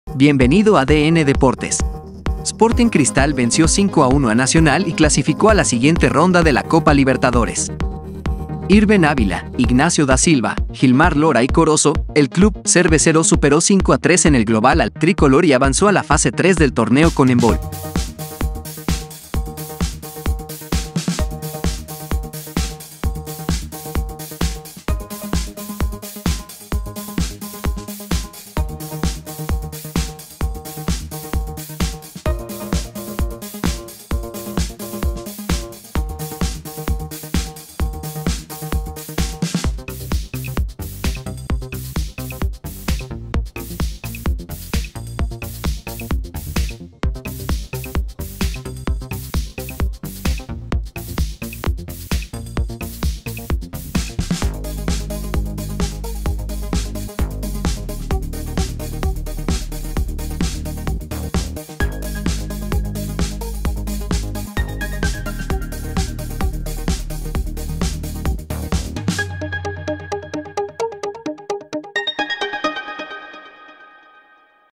¡Remontada soñada! Así fue el golazo de Jhilmar Lora que hizo explotar el estadio Nacional
El jugador de Sporting Cristal dejó un golpeo de balón extraordinario y quizá involuntario para poner en ventaja a los suyos en la eliminatoria.